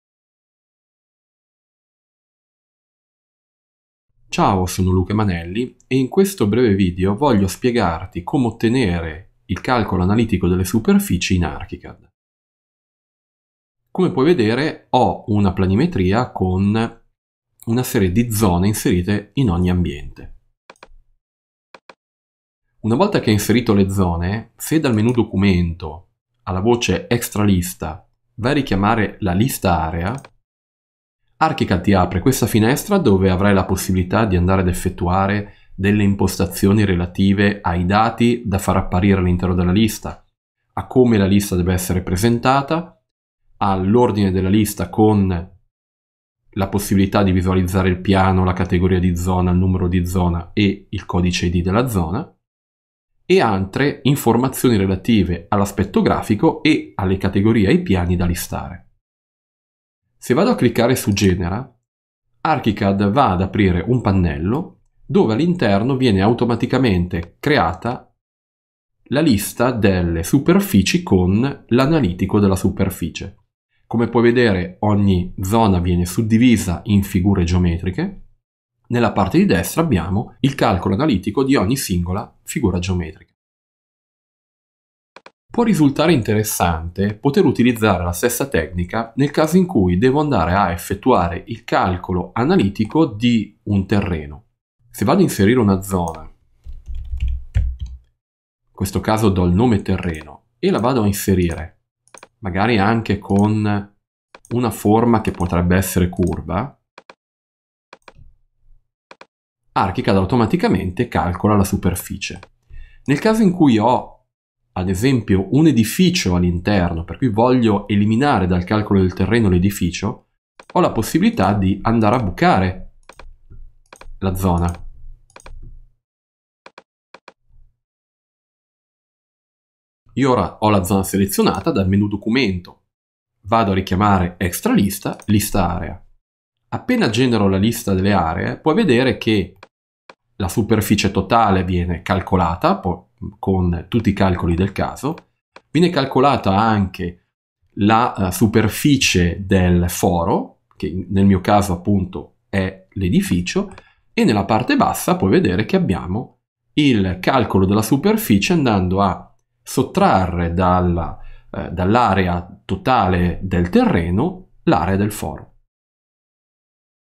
ARCHICAD Dimostrazione analitica della Superficie
La funzionalità descritta in questo video tutorial può essere utilizzata fino alla versione 21 di ARCHICAD.

La dimostrazione analitica di una superficie è una di quelle operazioni noiose e a volte particolarmente lunghe...seconda solo alla quotatura!
ARCHICAD ti mette a disposizione una interessante funzione che puoi sfruttare per velocizzare il tuo lavoro e che ti permette di ottenere una dimostrazione analitica delle superfici in pochi passaggi.

Se ne vuoi sapere di più puoi visitare il mio blog